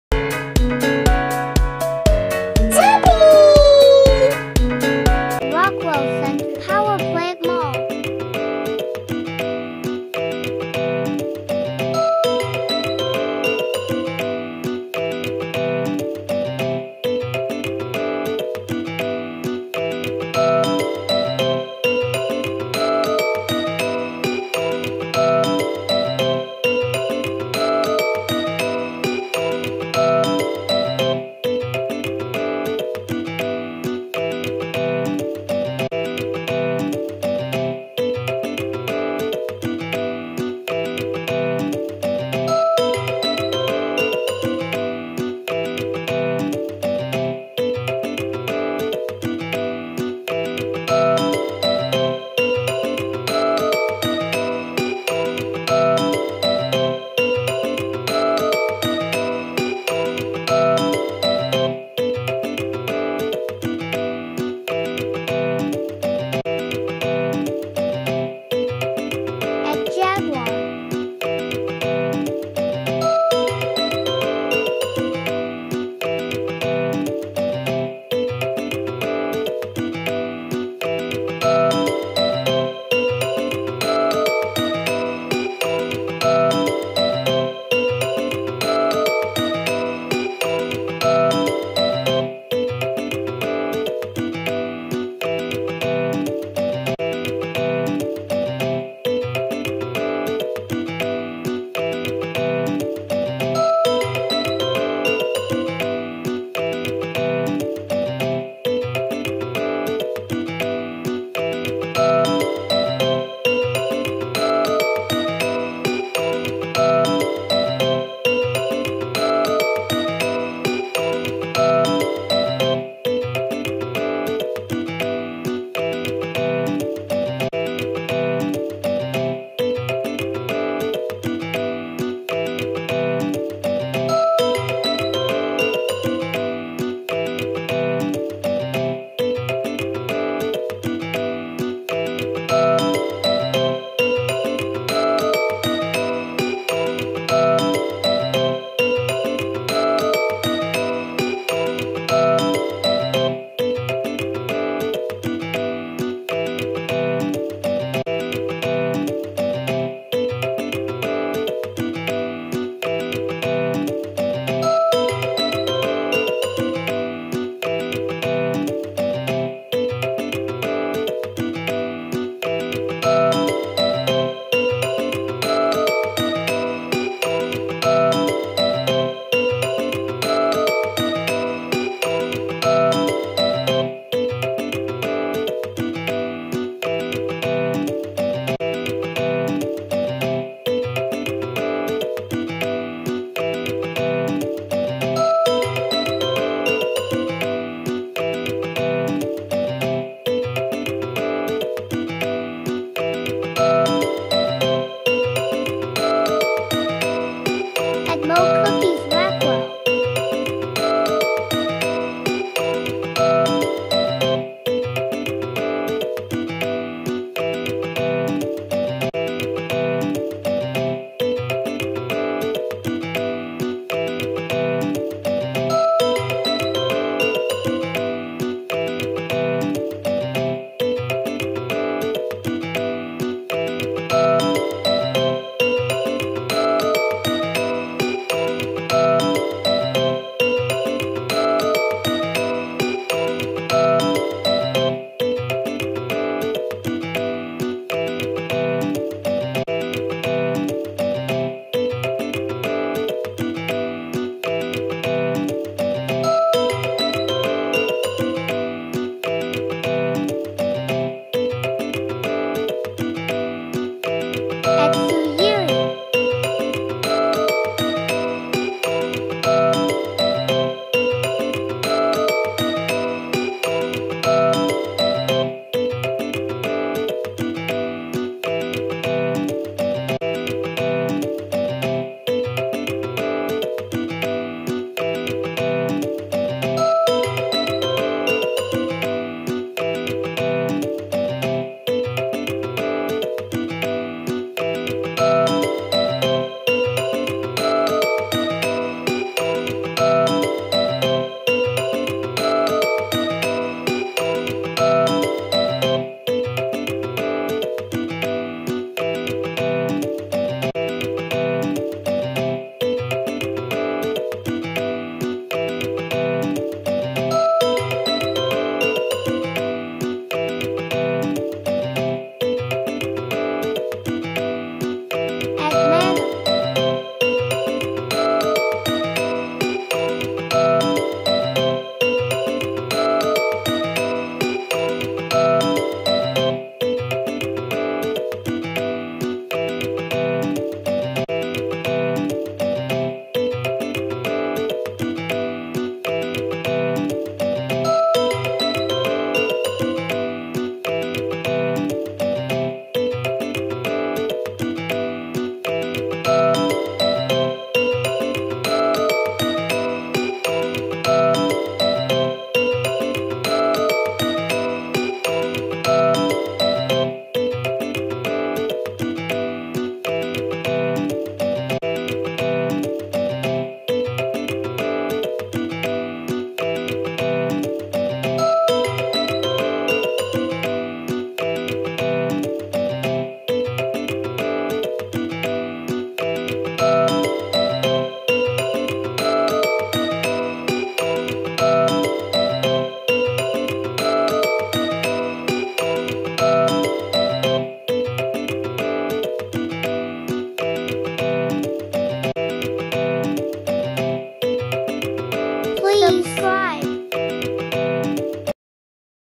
Zhappi tour in Rockwell Center Power Plant Mall Makati 2022 | MO' Cookies | Jaguar car
Tour in Rockwell Makati.
Some of the best food to eat in Rockwell Center Power plant Mall:
MO' Cookies
Tsujiri
Manam

Also, meet the Jaguar cars.